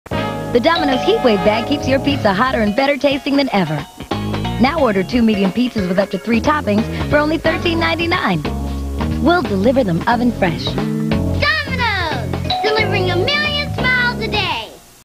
Domino's Pizza - Delivering A Million Smiles A Day (1990s) TV Commercial
1990s TV commercial for Domino's pizza delivery.